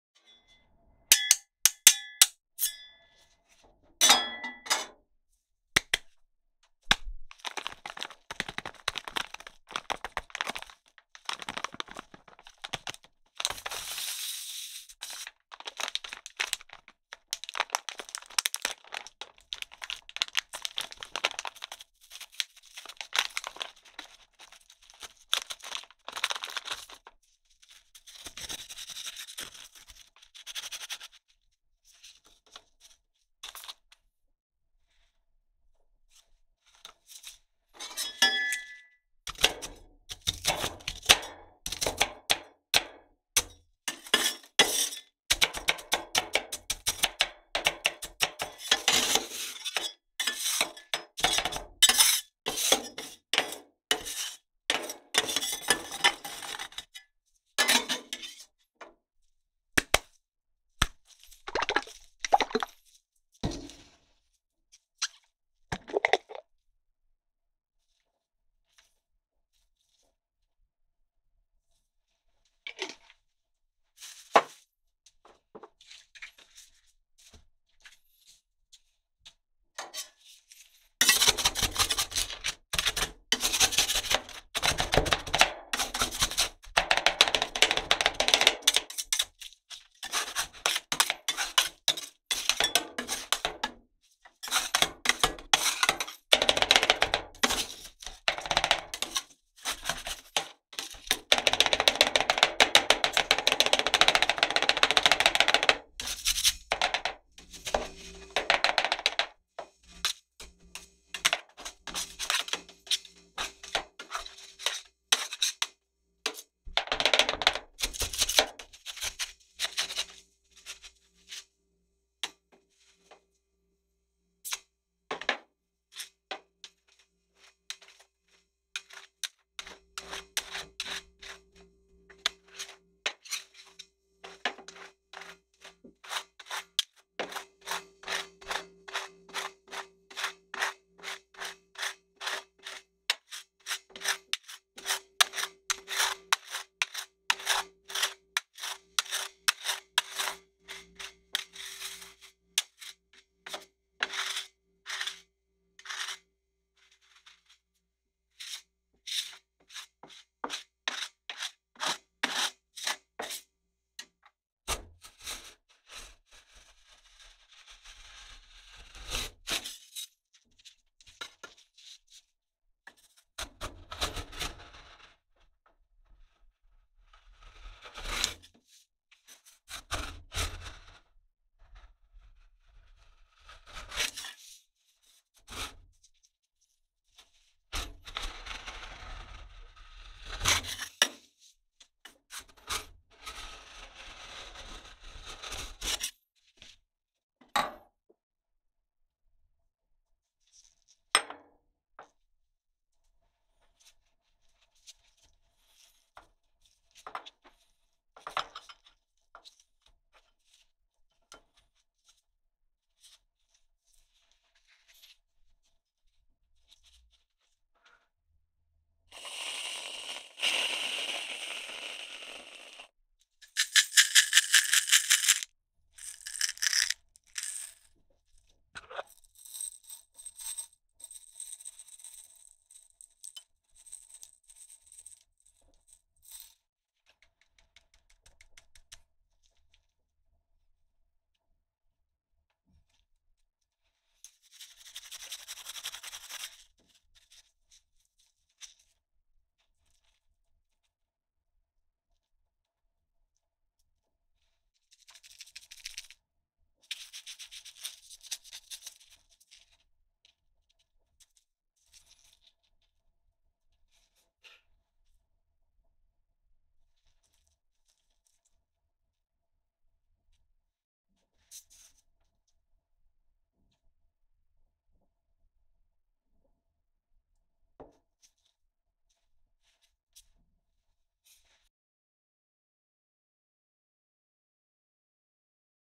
에이스로 아이스크림 롤 만들기 / ASMR / Making Iron Plate Ice Cream Rolls with "Ace"
"안녕하세요~
오늘은 한국 과자인 "에이스" 라는
과자로 아이스크림을 만드는 영상을 가져왔습니다

다음 영상을 기다려주세요~

hello~
Today, Korean sweets ""Ace""
I brought a video of making ice cream with sweets

Please enjoy the video today
Please wait for the next video ~"